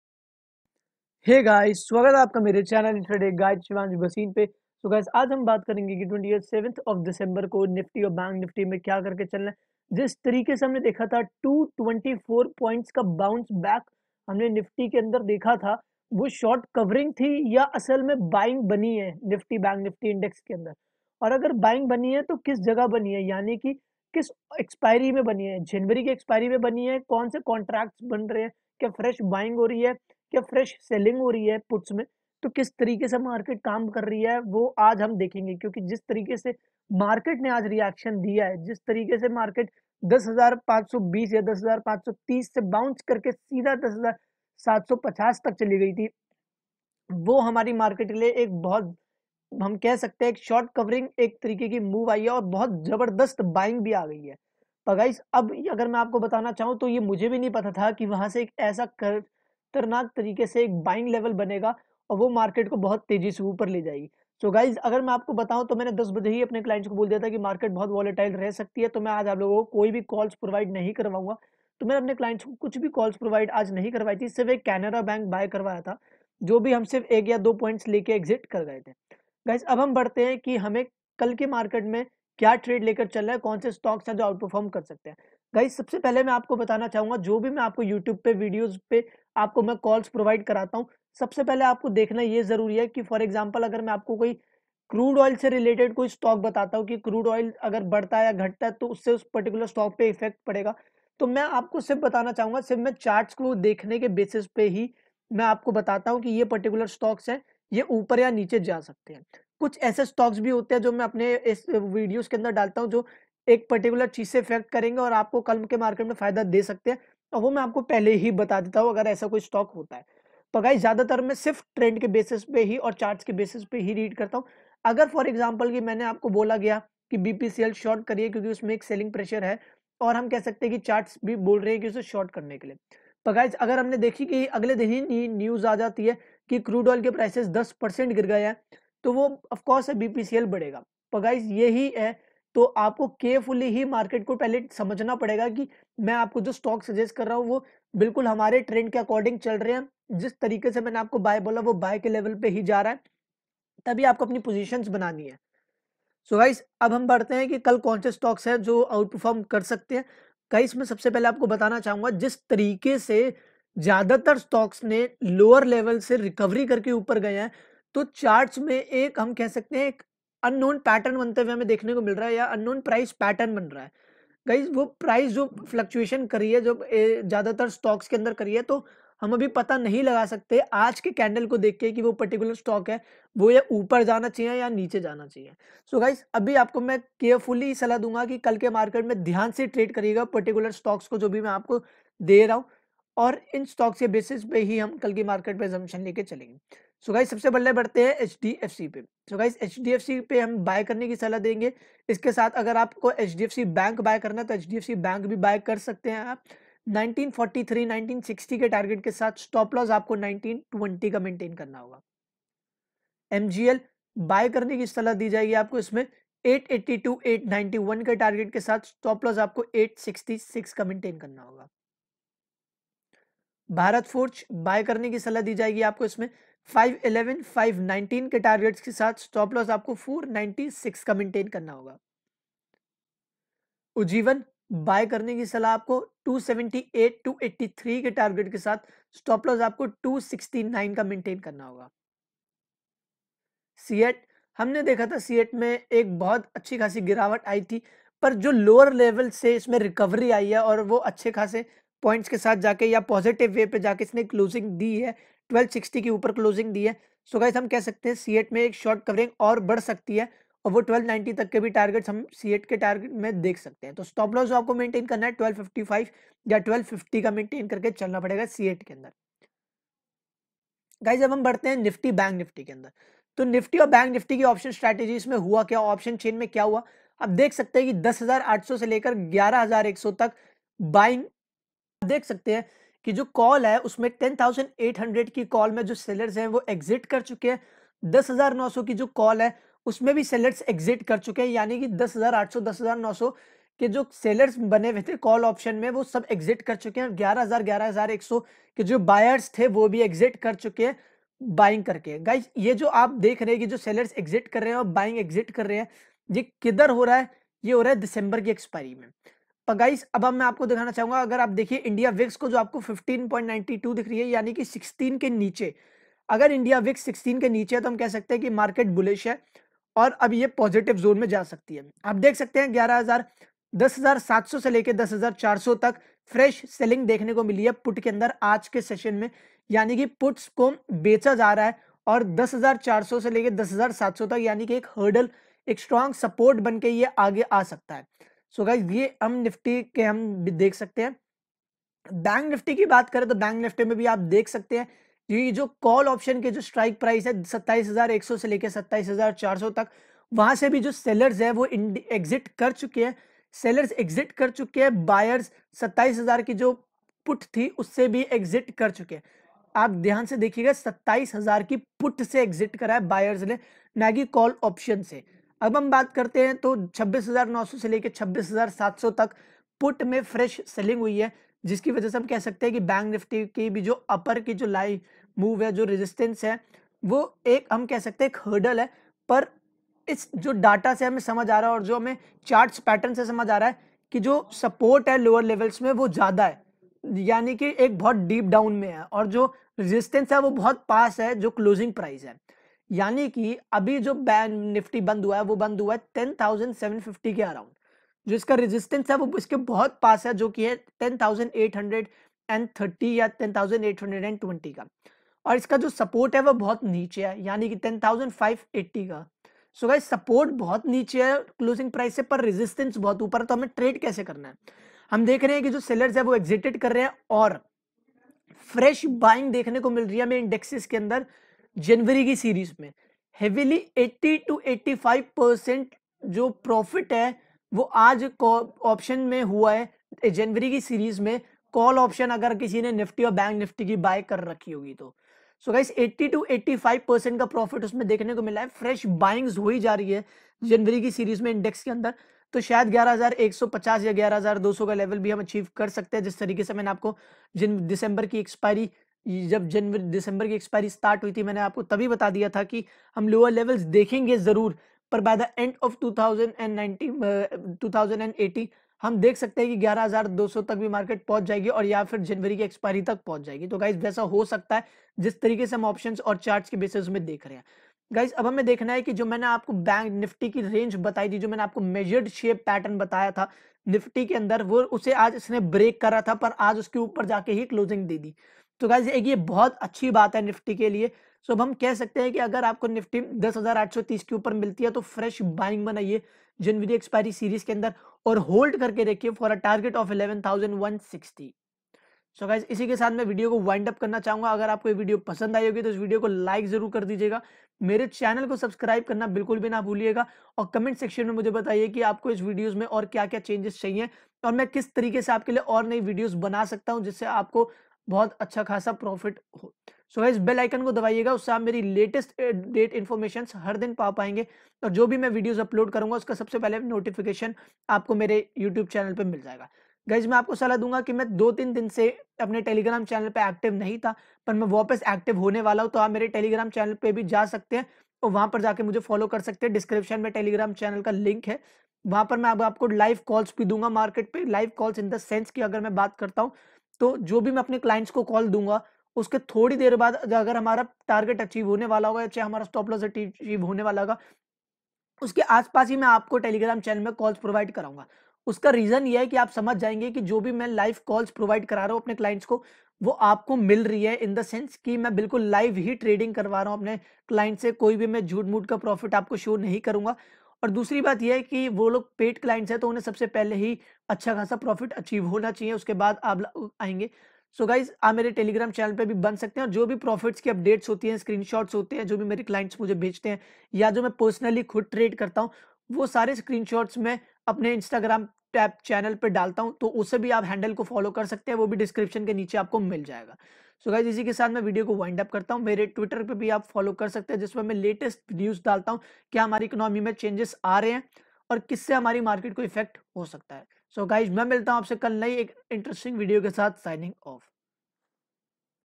#27 Dec.18 Nifty/Bank nifty OPTION TRADING.|| "5 stocks for Intraday i.e. 27 December 2018
WANT     "PAID INTRADAY SERVICE FOR FUTURES & OPTIONS"

live nifty/ bank nifty OPTION TRADING and technical analysis  for 27 Dec. 18. also option strategy for "NIFTY/BANK NIFTY "..

1. HDFC

2. CEAT

3. MGL

4. BHARAT FORGE

5. UJJIVAN

 by using indicators of: pivot points, moving averages, stocastic momentum index. maintain your strict stop loss and enjoyably achieve your targets.

                     intradayguide

YOUTUBE.COM/C/INTRADAYGUIDE